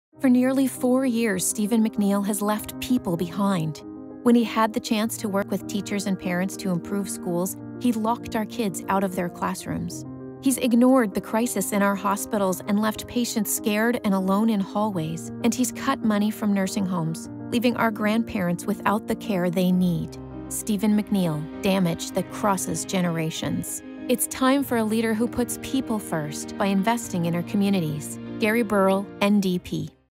Damage that Crosses Generations
For nearly four years, Stephen McNeil has left people behind.

When he had the chance to work with teachers and parents to improve schools, he locked our kids out of their classrooms.

He’s ignored the crisis in our hospitals and left patients scared and alone in hallways.

And he’s cut money from nursing homes, leaving our grandparents without the care they need.

Stephen McNeil. Damage that crosses generations.

It’s time for a leader who puts people first by investing in our communities.

Gary Burrill. NDP.